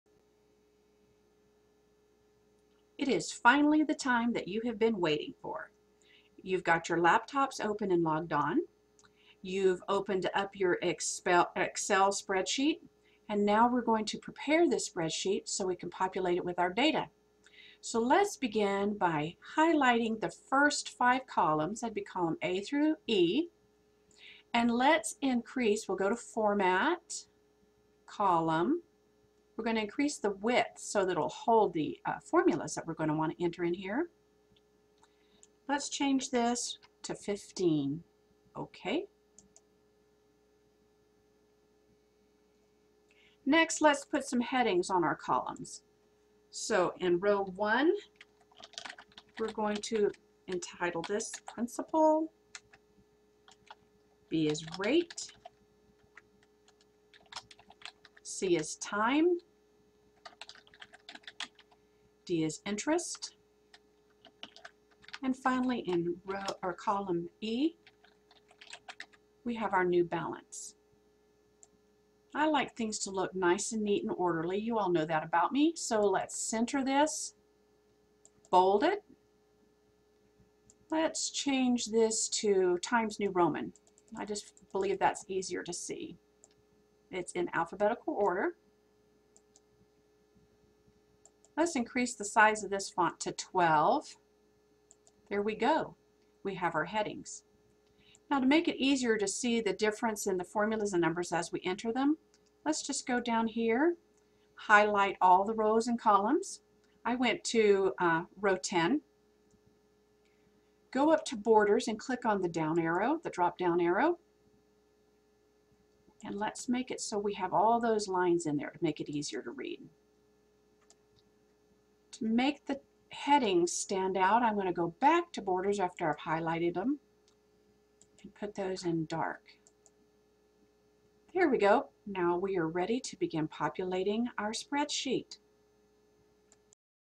Simple Interest Spreadsheet
How to design a simple interest Excel spreadsheet